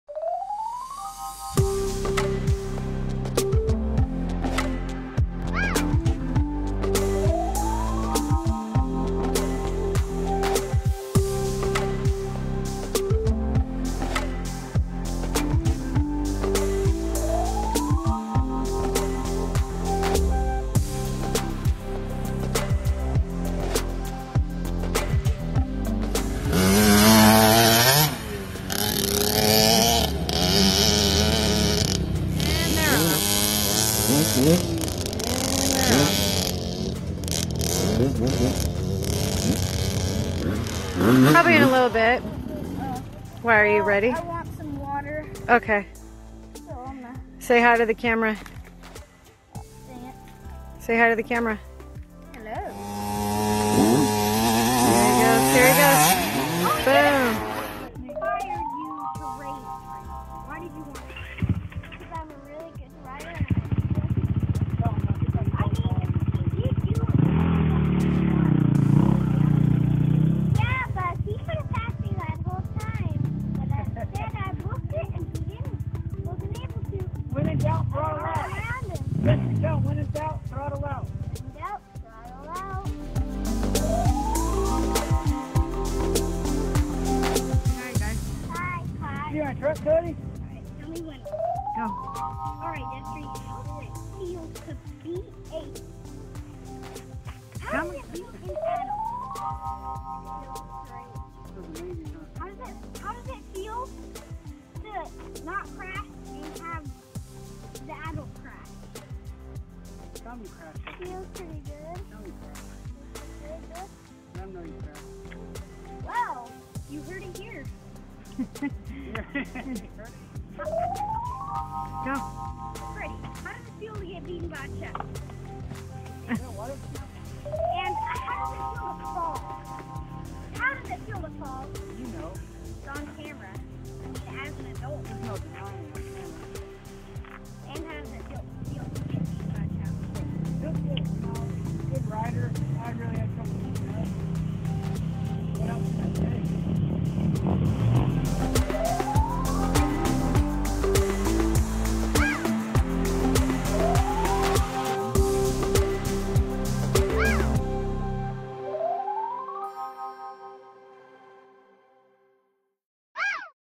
LITTLE KTM RIDERS
The boys are fond of riding their ktm dirtbikes. They love doing it whever they have time.